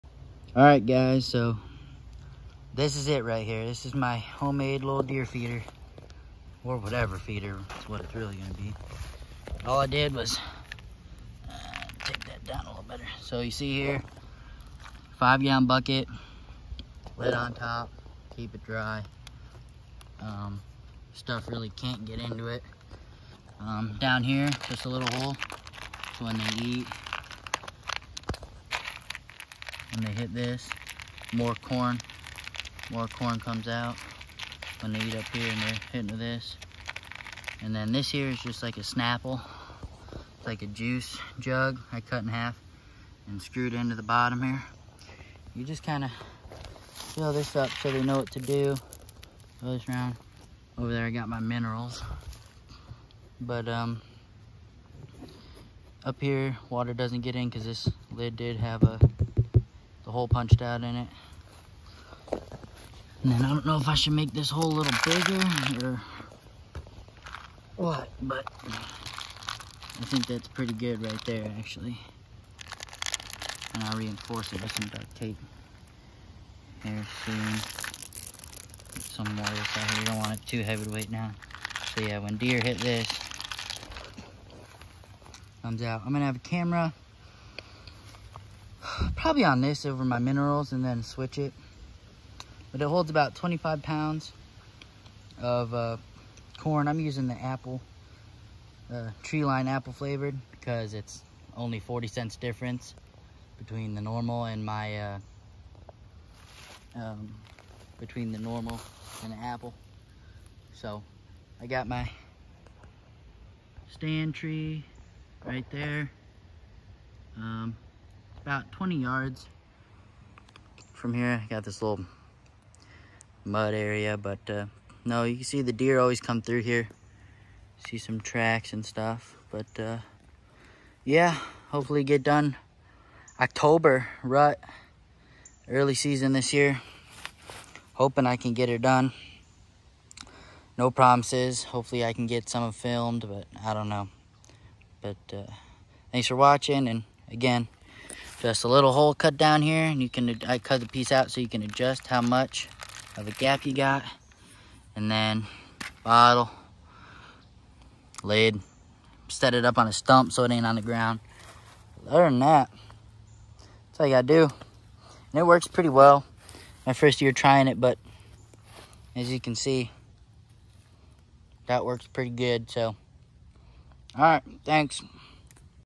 Homemade deer feeder
Homemade deer feeder out of 5 gal bucket